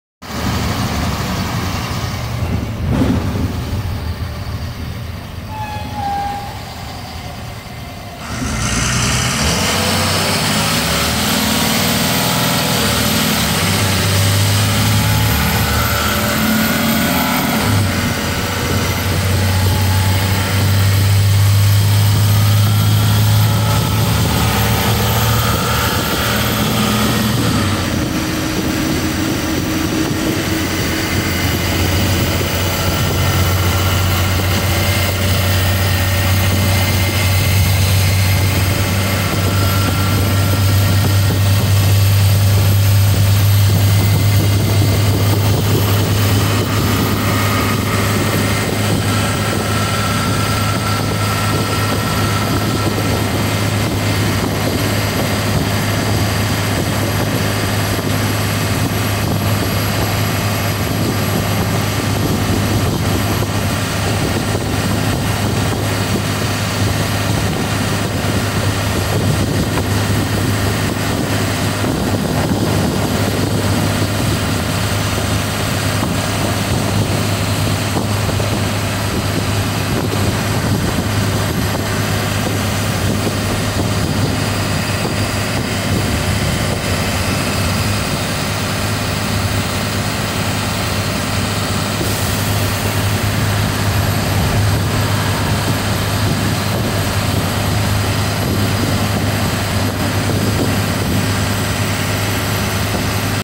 Highway Acceleration on 2005 Thomas EF School Bus
This is the fastest this bus will go! It has a governor on the engine but it is a very great bus. Runs the best out of the whole fleet!

Cummins power! 💪